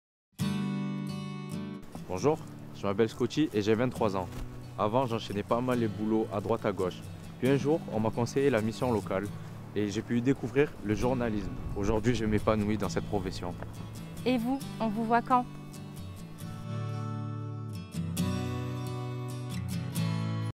Scotty LAB'ON-ID 2020
Grâce à la Mission Locale du Pays Salonais, Scotty qui n'avait pas de véritable projet professionnel a pu devenir journaliste. Et vous ? OnVousVoitQuand ?